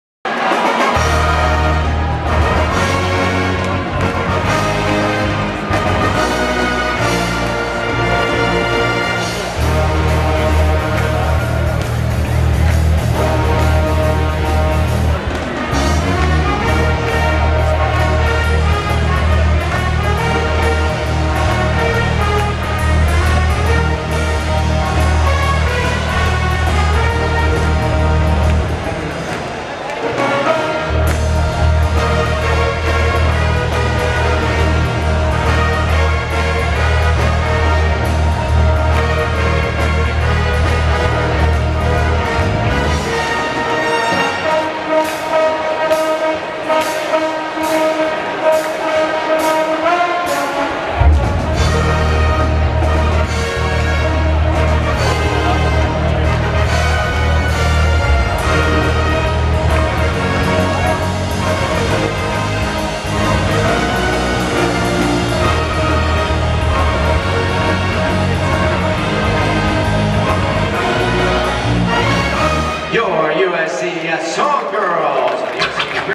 USC Song Girls - Halftime performance USC vs Colorado 1/10/2018
USC Song Girls perform during halftime of USC vs Colorado men's basketball game 1/10/2018

USC Song Girls, USC Cheerleaders, USC Trojans